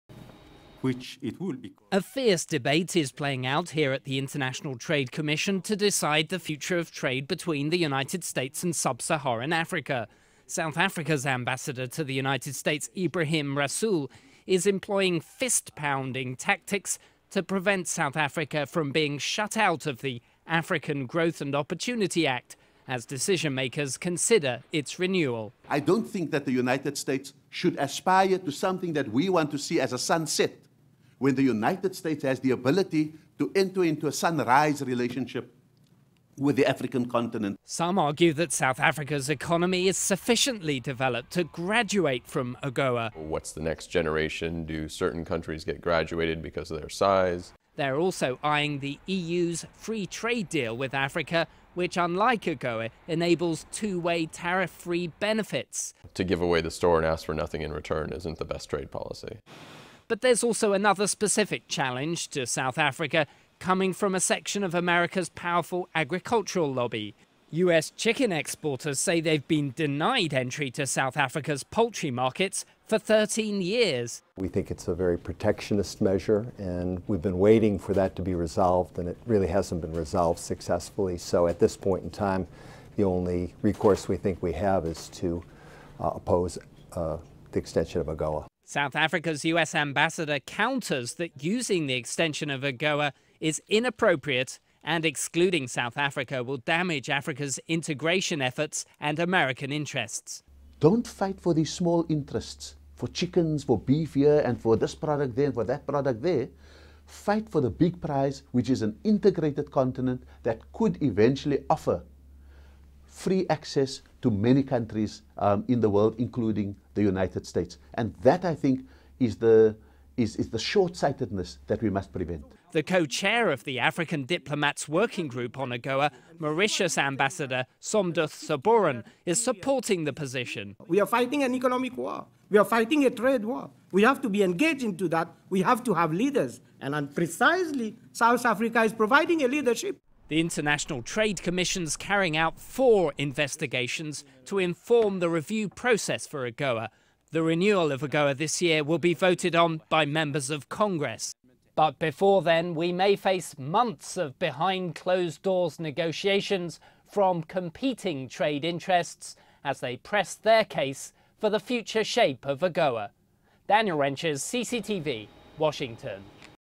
Future of AGOA; South Africa battles to avoid being shut out of preferential American market
A fierce debate is playing out here at the International Trade Commission to decide the future of trade between the United States and sub-Saharan Africa. South Africa's Ambassador to the United States Ebrahim Rasool is employing fist-pounding tactics prevent South Africa from being shut out of the African Growth and Opportunity Act as decision makers consider its renewal.